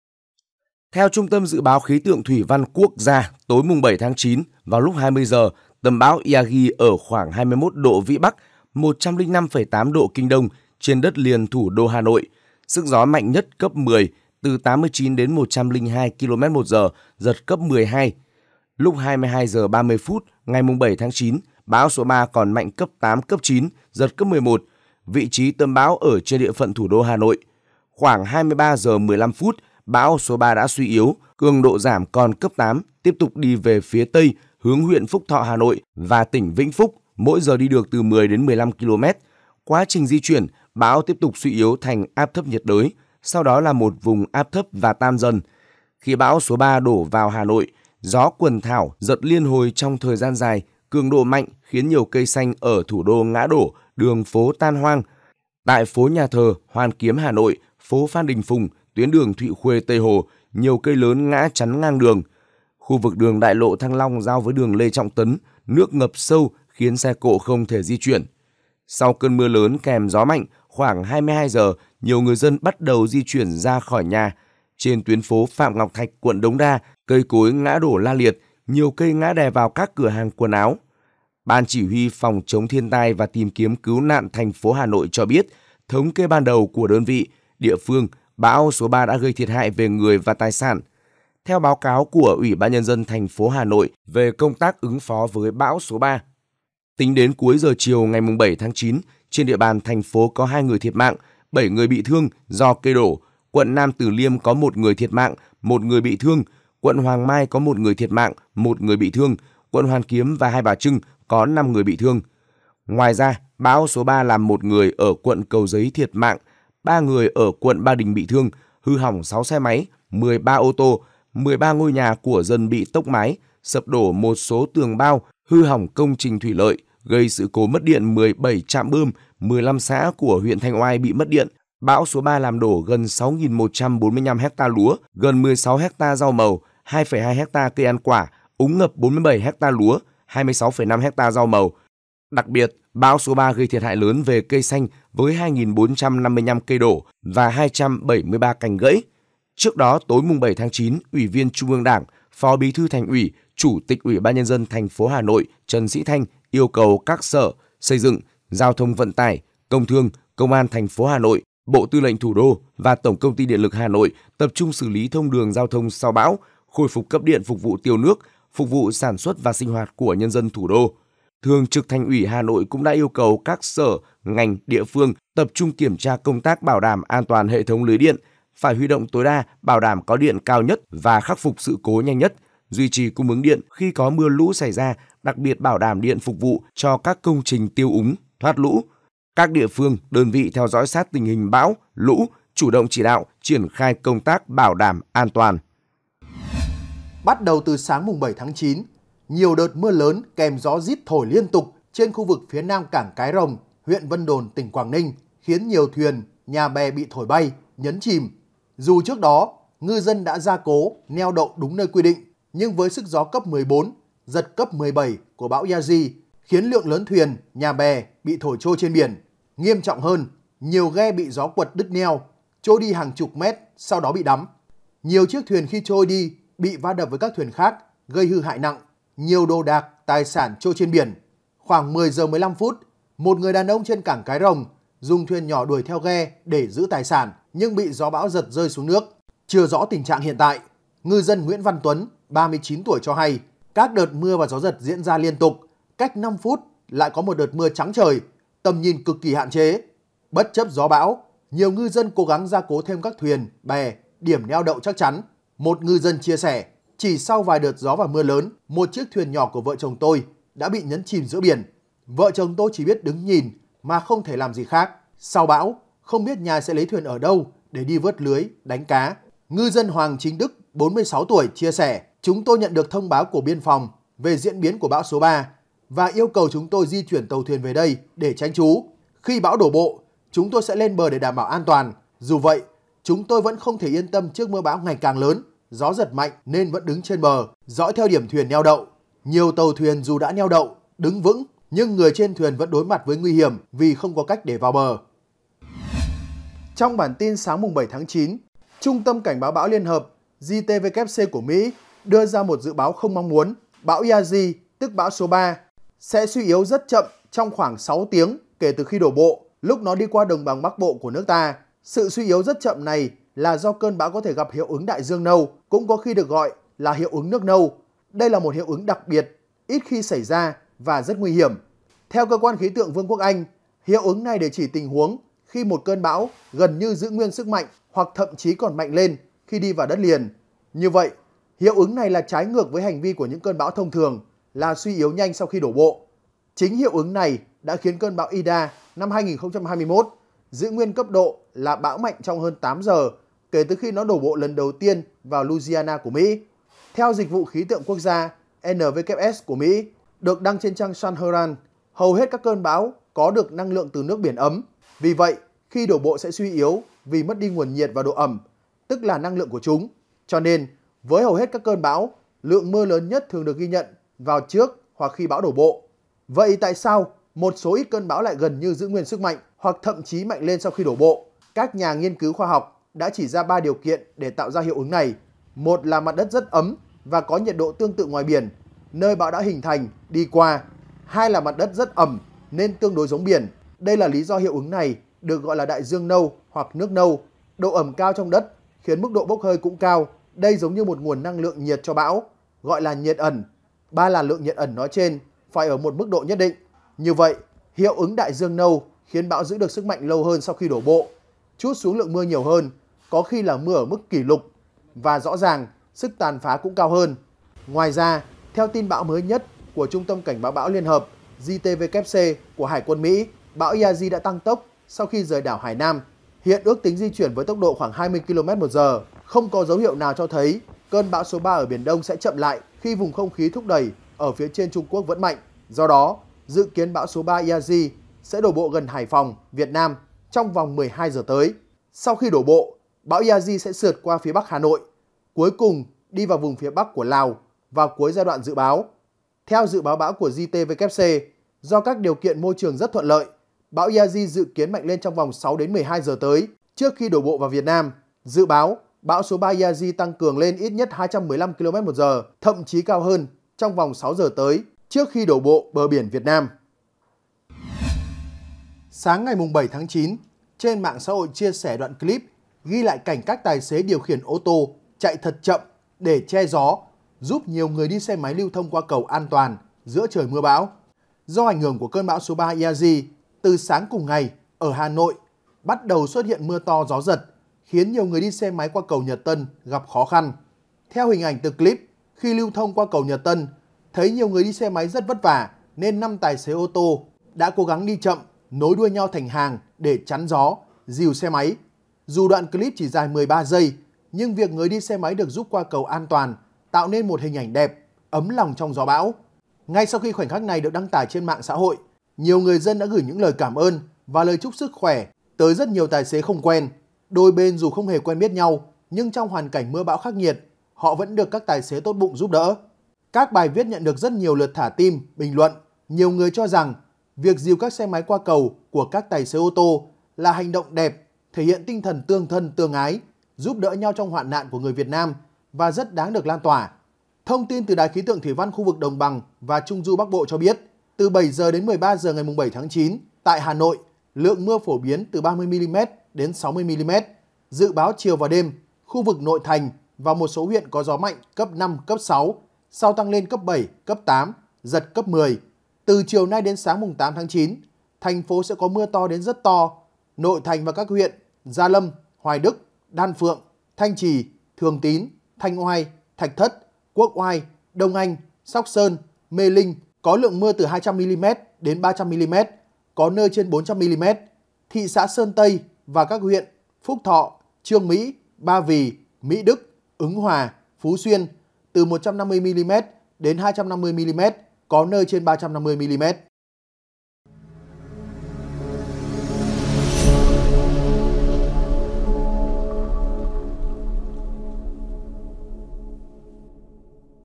🔥BẢN TIN: BÃO SỐ 3 QUẦN THẢO HÀ NỘI CẢ ĐÊM, GIÓ GẦM RÍT KHỦNG KHIẾP, ĐƯỜNG PHỐ TAN HOANG KHẮP NƠI
Hà Tĩnh TV | Bản tin: Bão số 3 quần thảo Hà Nội cả đêm, gió gầm rít khủng khiếp, đường phố tan hoang khắp nơi
Theo Trung tâm Dự báo khí tượng thủy văn Quốc gia, tối 7/9, vào lúc 20h, tâm bão Yagi ở khoảng 21.0 độ Vĩ Bắc; 105.8 độ Kinh Đông, trên đất liền Thủ đô Hà Nội. Sức gió mạnh nhất: Cấp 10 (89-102km/h), giật cấp 12.